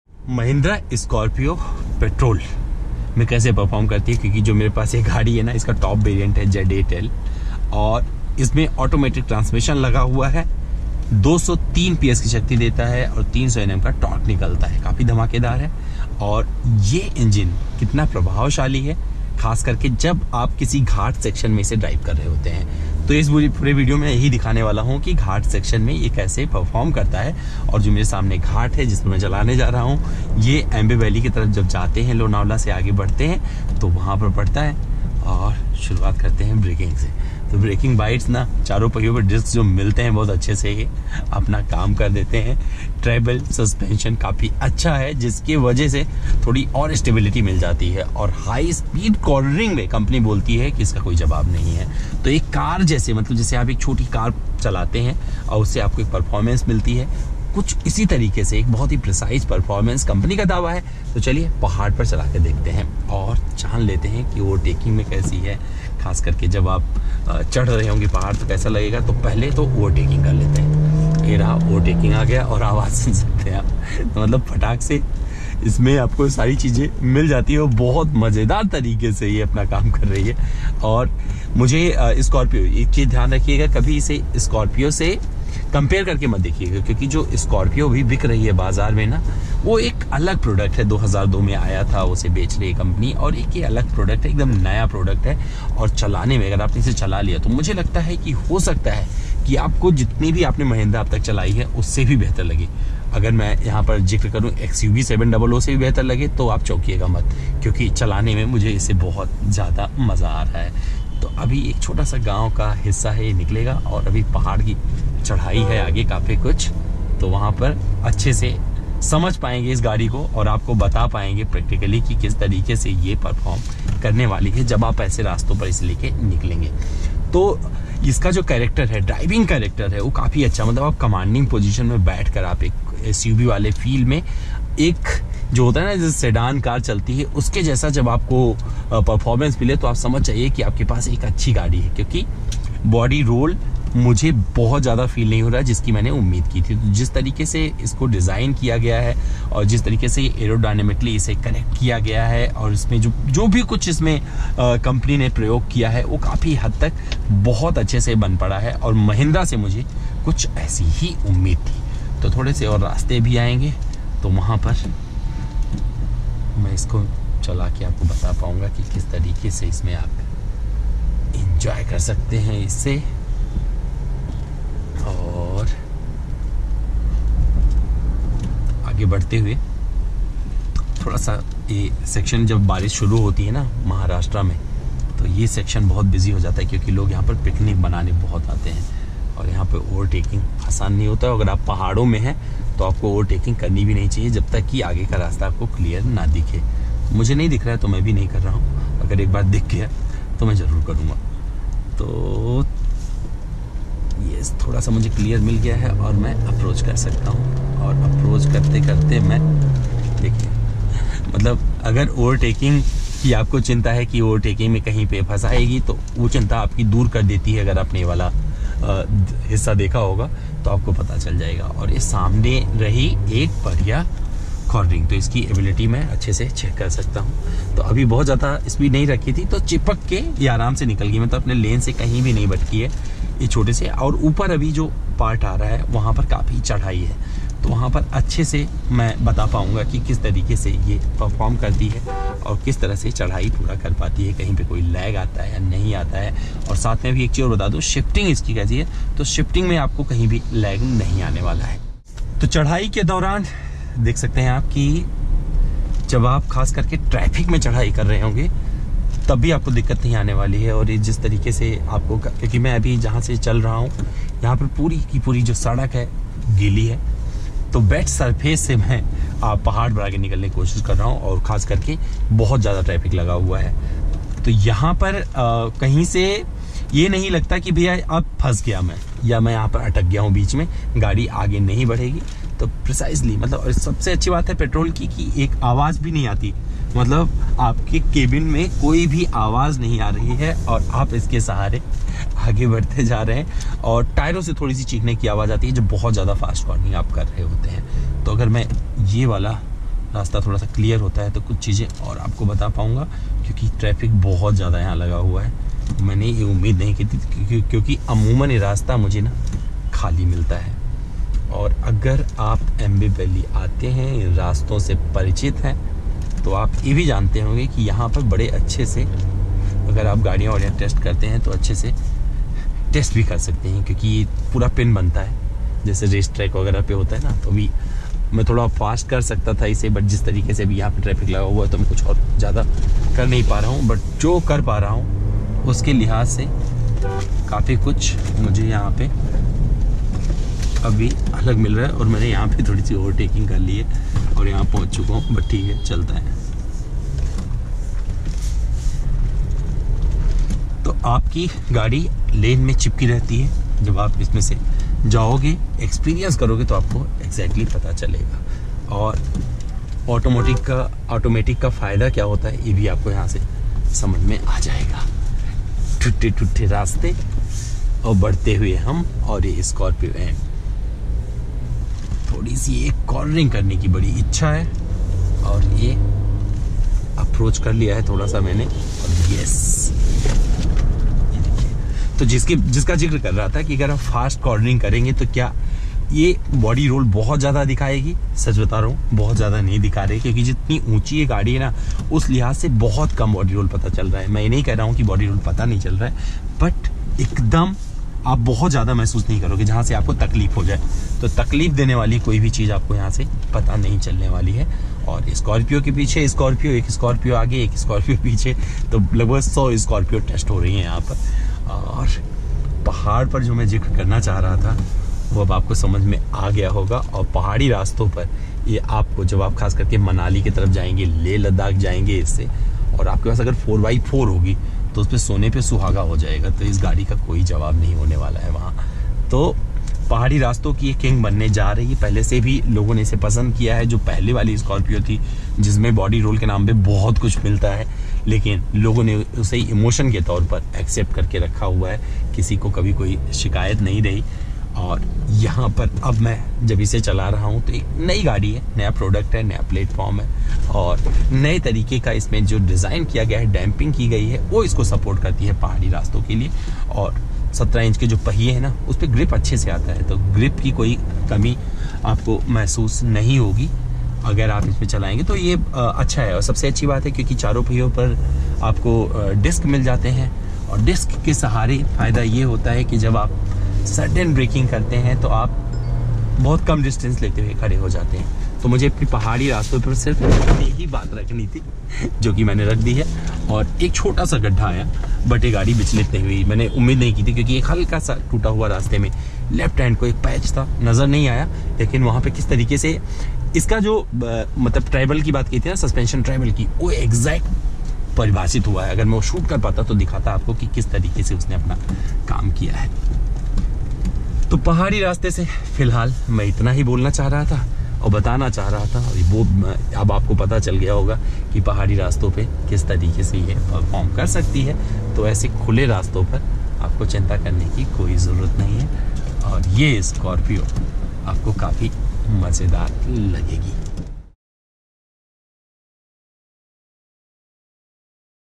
Mahindra Scorpio N का पहाड़ी परीक्षण || Hill Drive Review || Power On Wheel
आज के इस वीडियो में हम आपको बताएंगे कि Mahindra Scorpio N Petrol AT पहाड़ी रास्तों पर कैसा Perform करती है. वीडियो अंत तक ज़रूर देखिएगा..